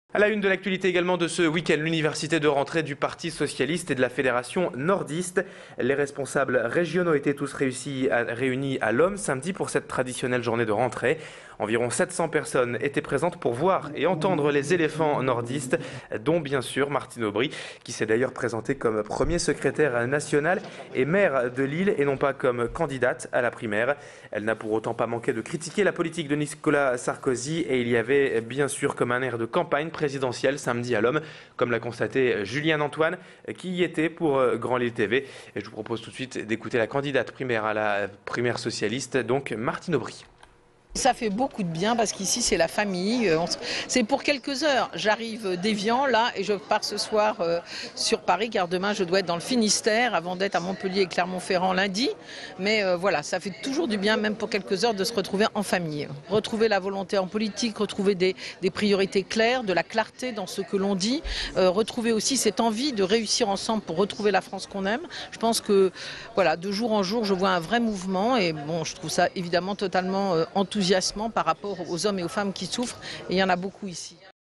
Université PS : Le parti socialiste fait sa rentrée à Lomme
Comme chaque année, le PS du Nord marquait le coup pour la rentrée politique. Martine Aubry et Harlem Désir ont répondu présents

Sites webs/blogs, copiez cette vidéo avec le bouton "Intégrer" !